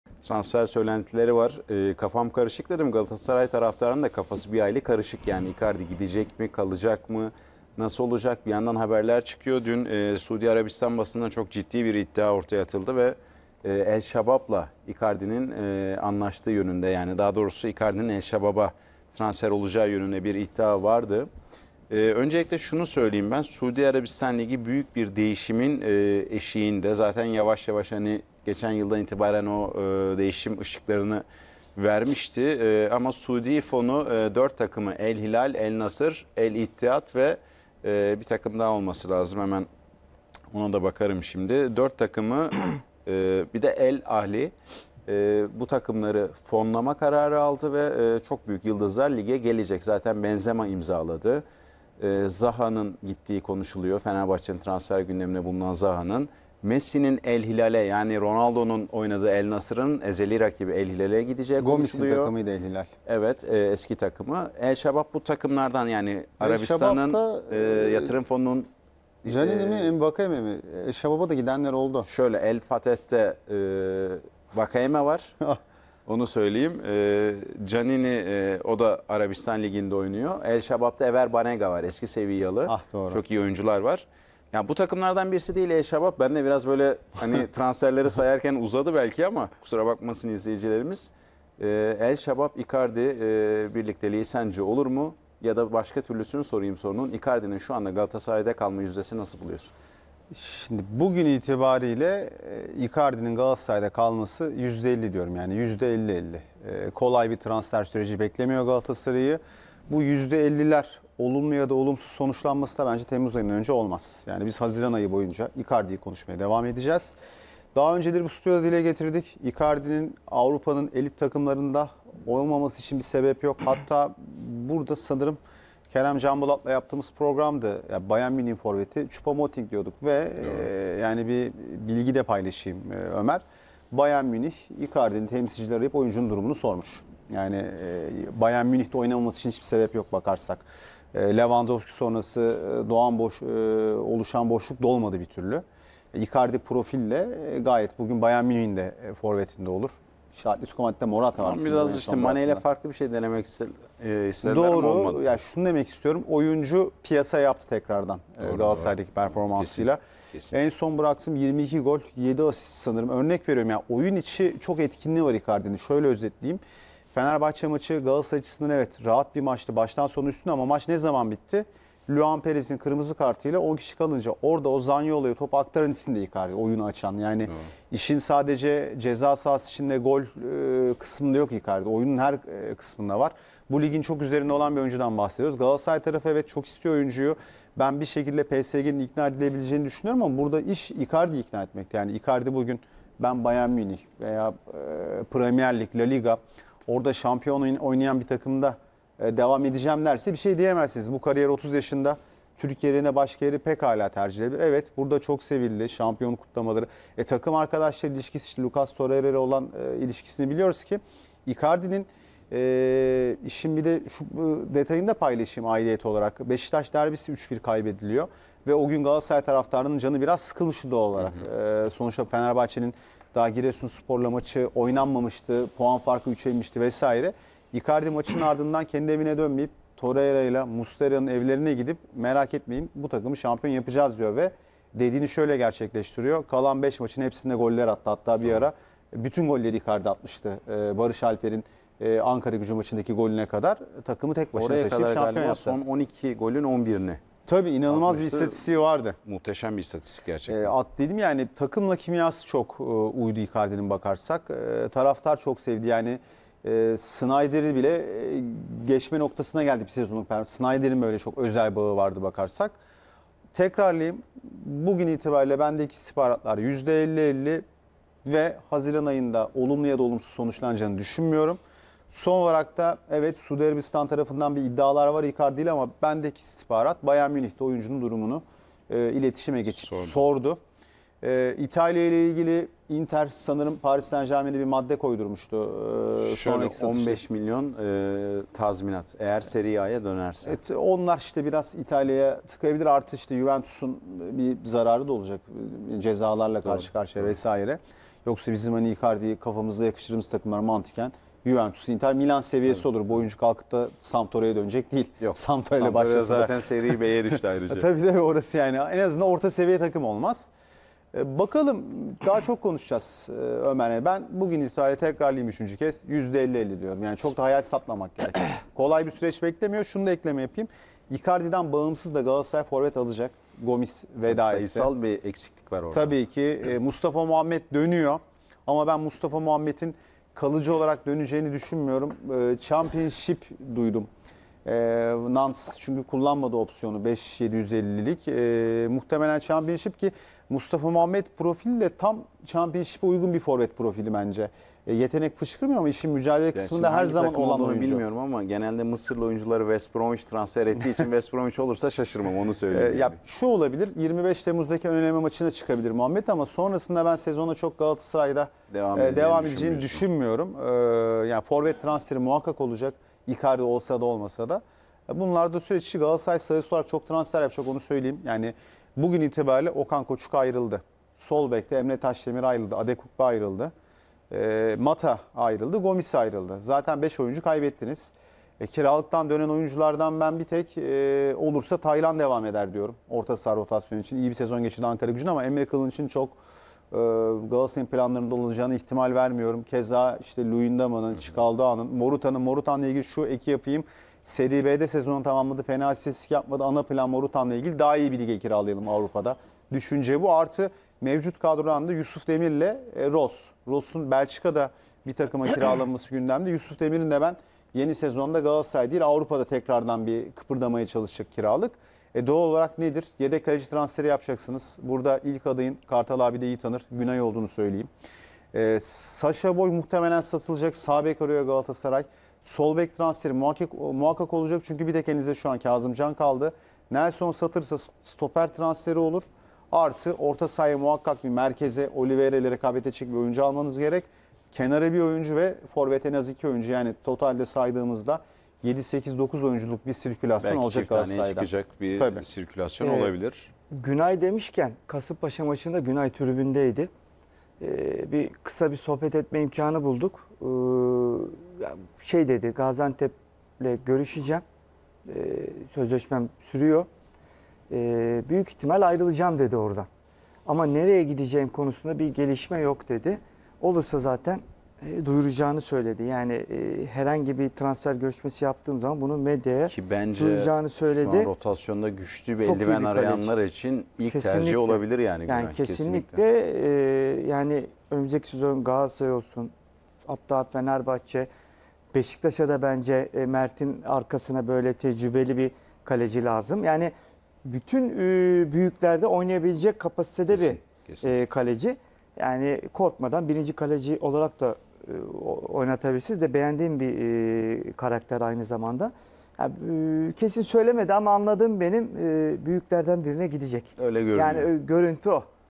Mehmet Özcan: "Bayern Münih Icardi'nin Durumunu Sordu, Transferde Devreye Girebilir" / A Spor
Mehmet Özcan: "Bayern Münih Icardi'nin Durumunu Sordu, Transferde Devreye Girebilir" / A Spor / Spor Ajansı / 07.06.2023